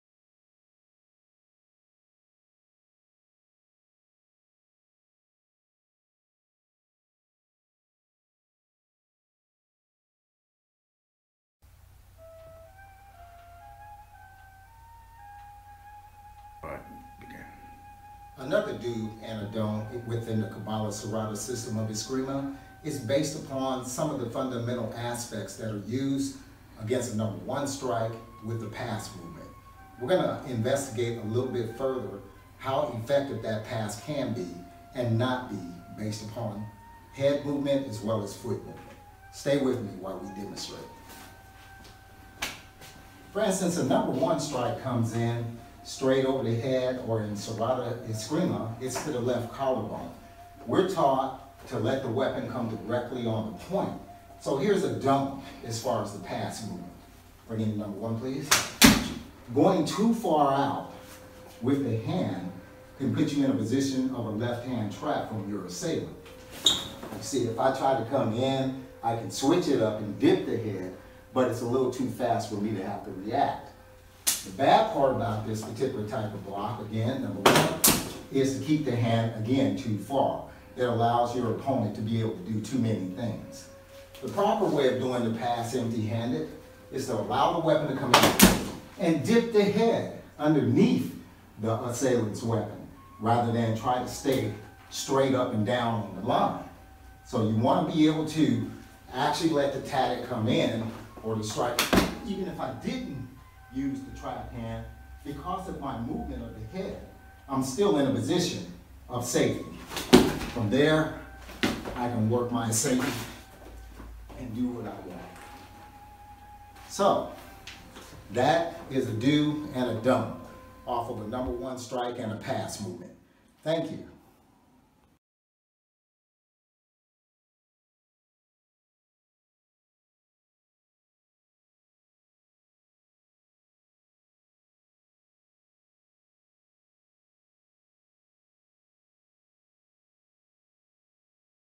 DO'S & DONT'S of Cabales Serrada Escrima (The Outside Pass Disected)
The "CSE Outside Pass" is one of the first evasive counter-strike tactics that is taught within an aspirants first class. Once the primary fundamentals which lies behind the pass movement have been efficiently mastered; the tactic can then be effectively applied within an actual self-defense situation.